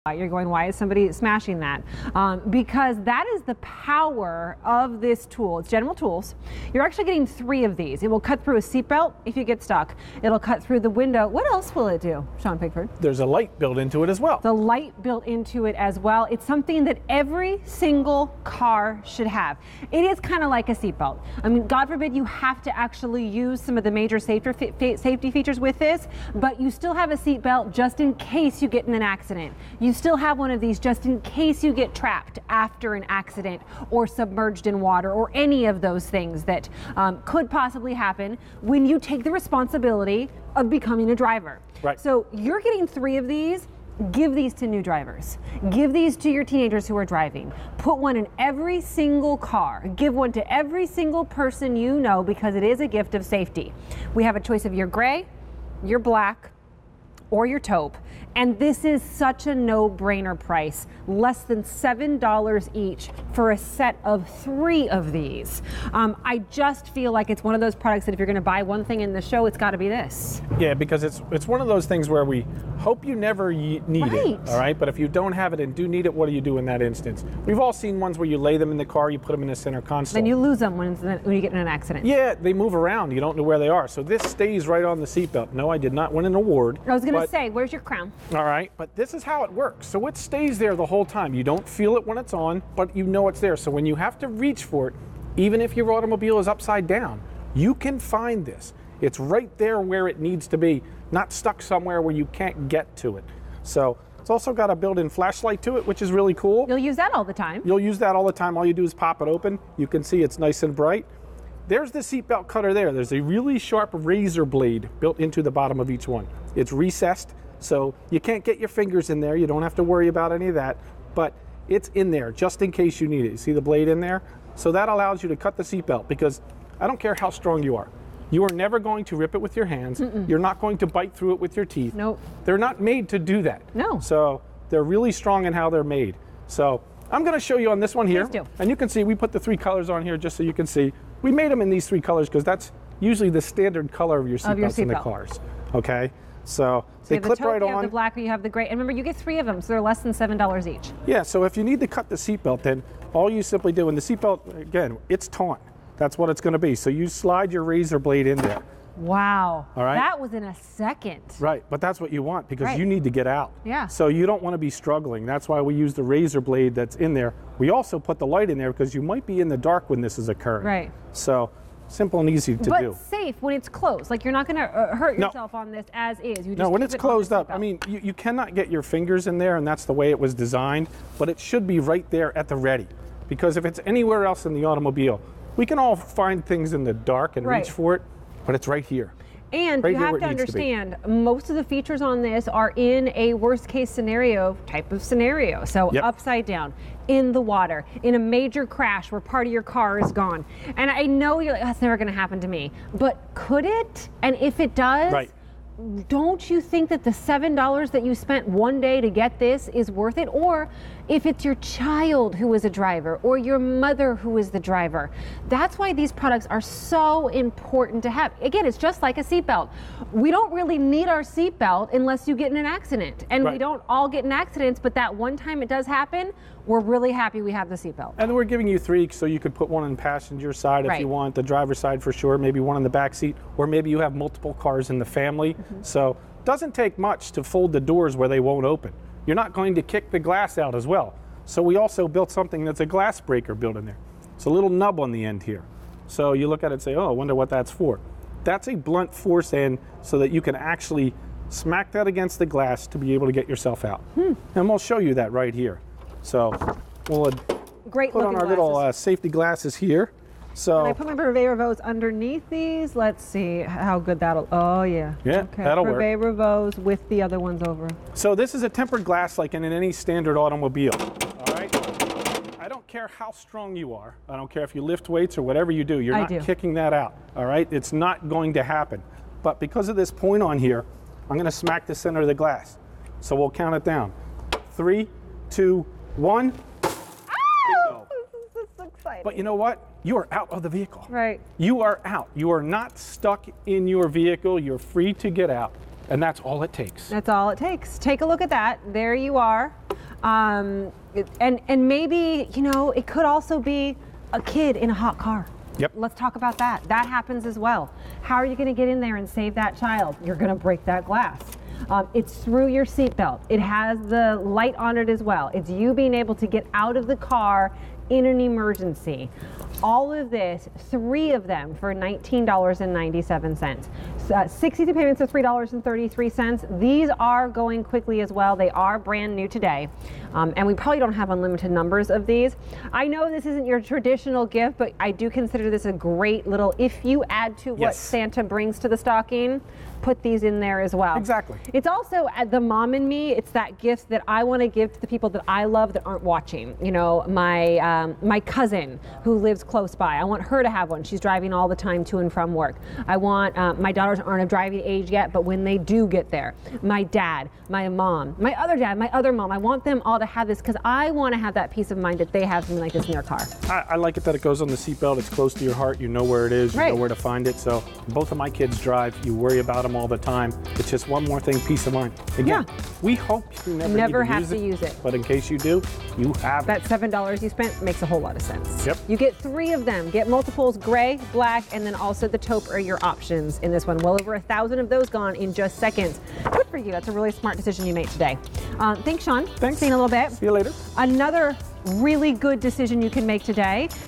General Tools S/3 Car Safety Tools with Seat Belt Clip and Gift Boxes on QVC
General Tools S/3 Car Safety Tools with Seat Belt Clip and Gift Boxes

Drive smart; drive safe. Each of these sets holds three tools: window breaker, seatbelt cutter, and flashlight. From General Tools.